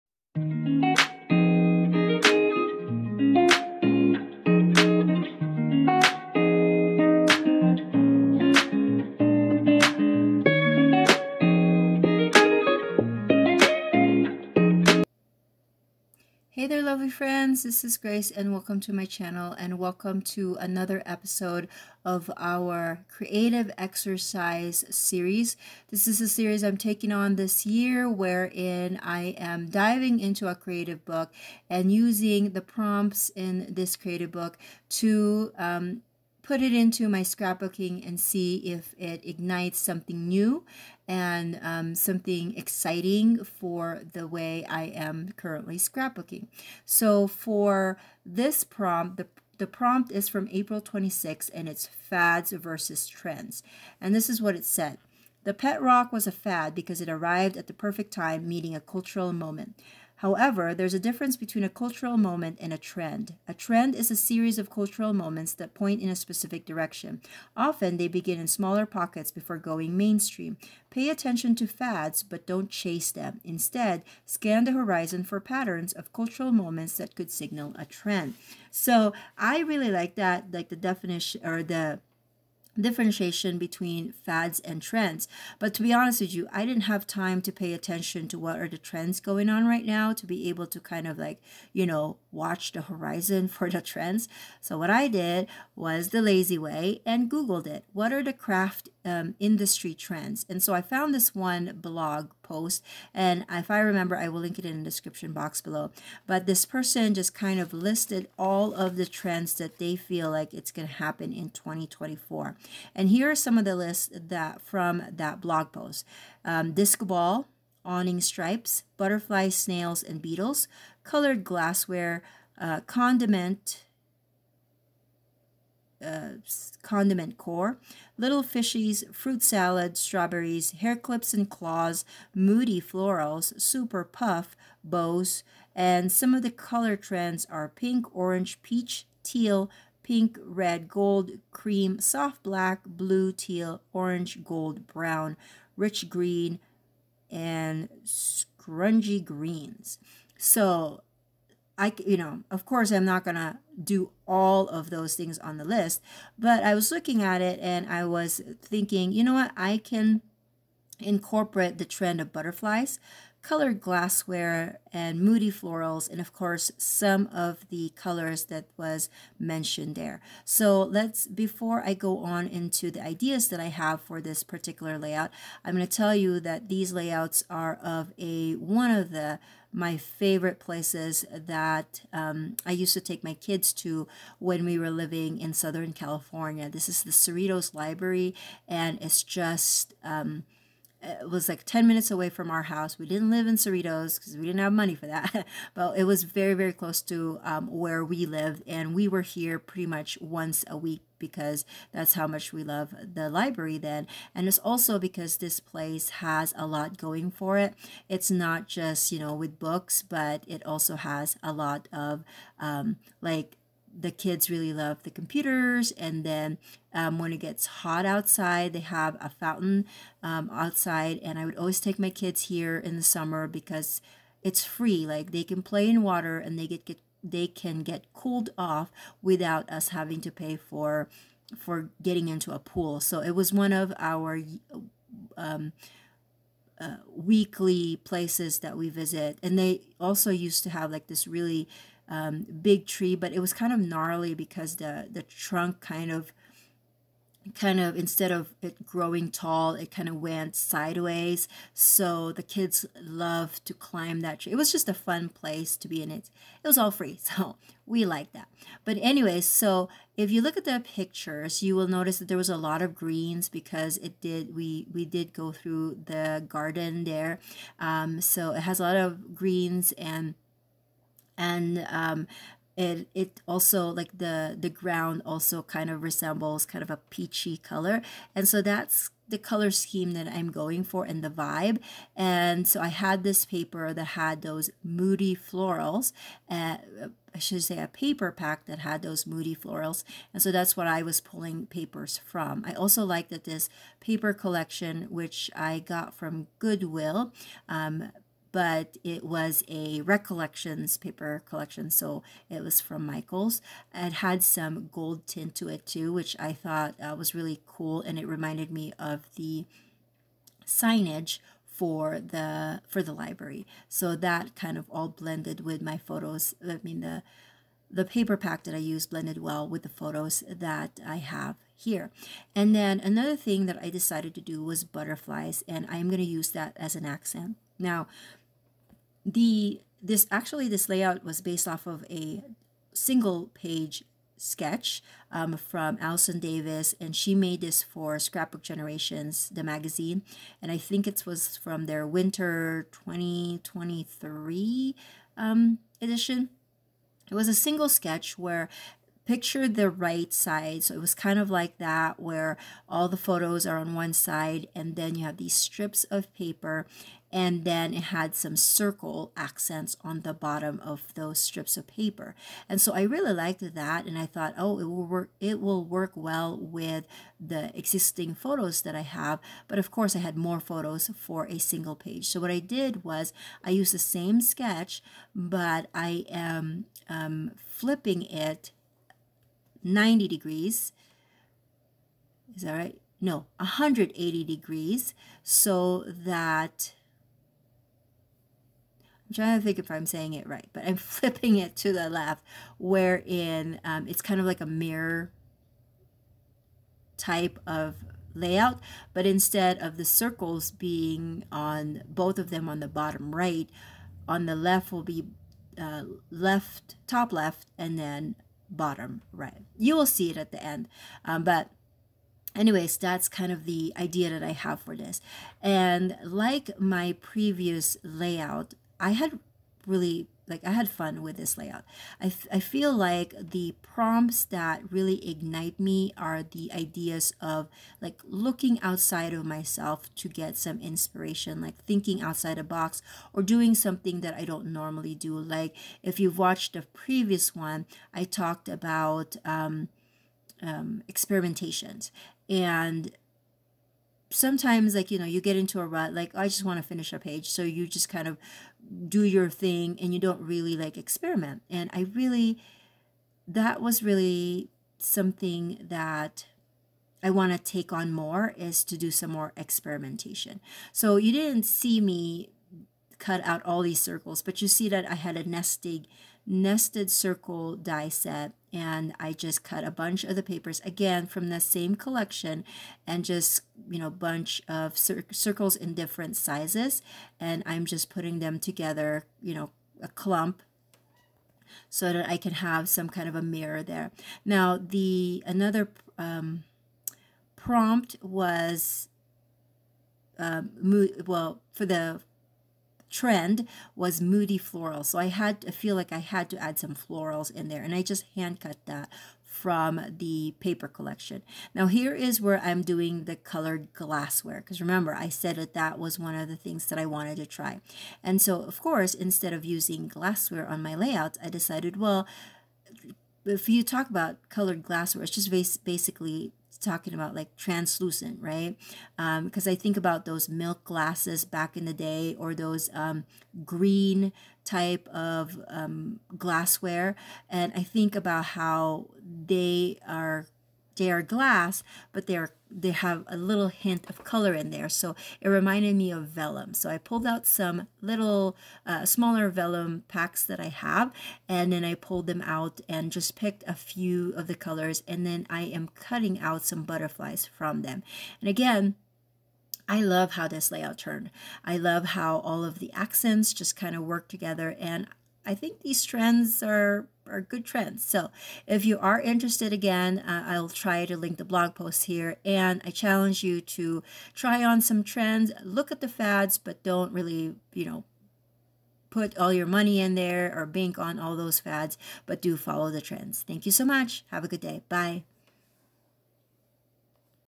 Creative Exercise Layout #8
Come join me as I go through the “Daily Creative” book by Todd Henry and view my scrapbook process through the lens of a creative. Come along as I discover ideas that might help me think outside the box and keep the creative spark going